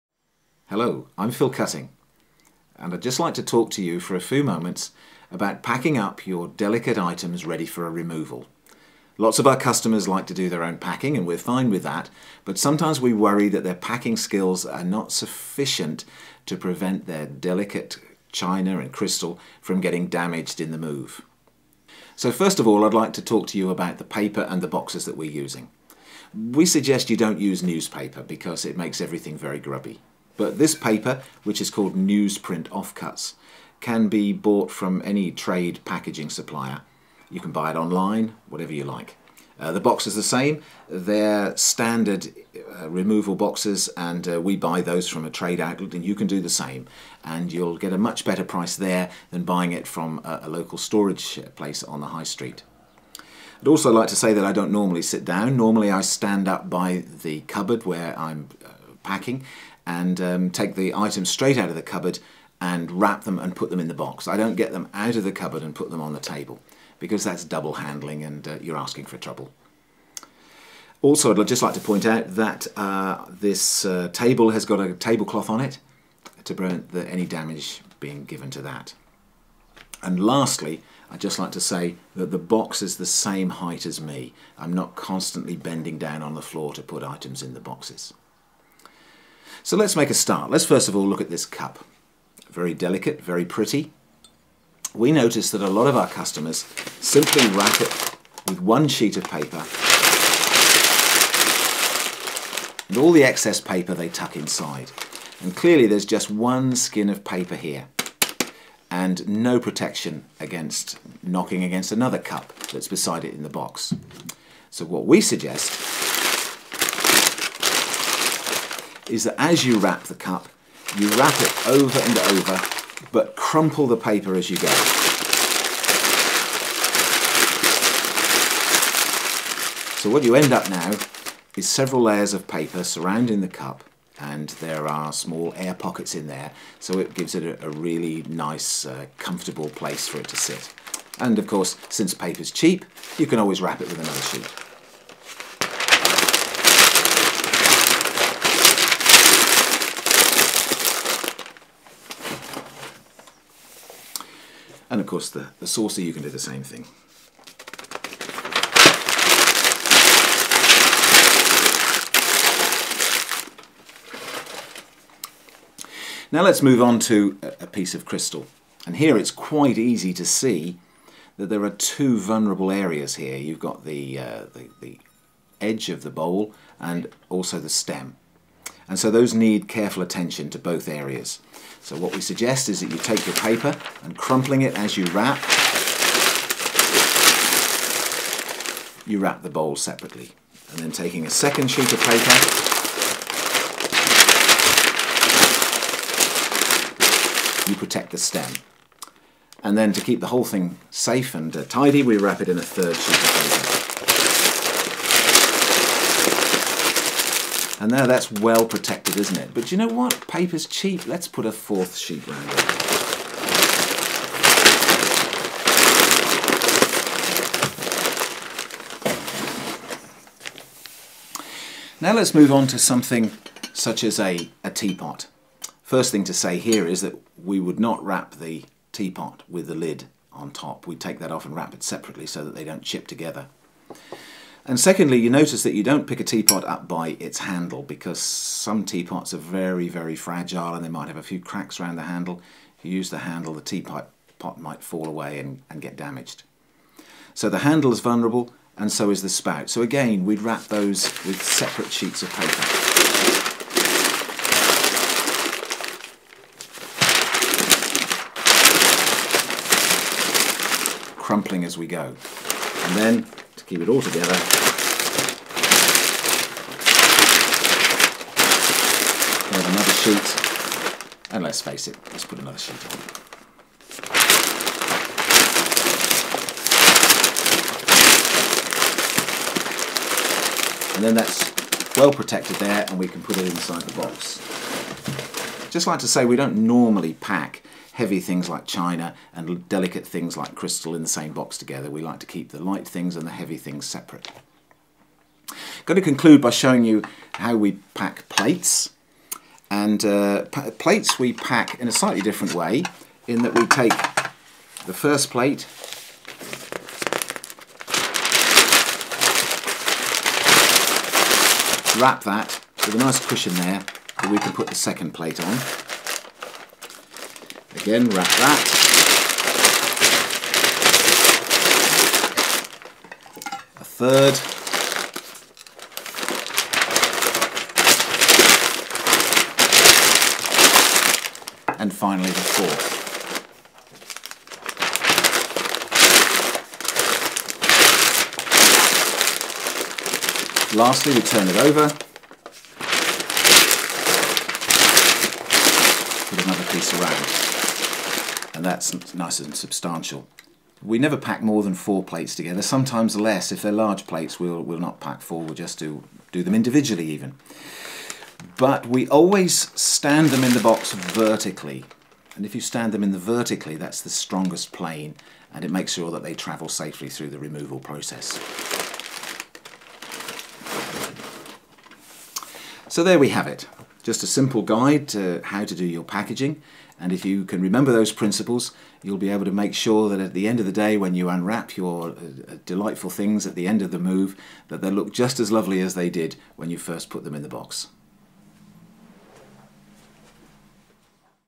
How To Pack China & Crystal When Moving House
Many of our clients like to pack their own delicate items but we frequently worry that their techniques leave a little to be desired. Unfortunately, many 'how to pack' videos on YouTube demonstrate that other professionals do not pack delicates to a high enough standard! In view of this, we decided to create our own guide. We hope you find it useful, but you can always ask us to pack on your behalf!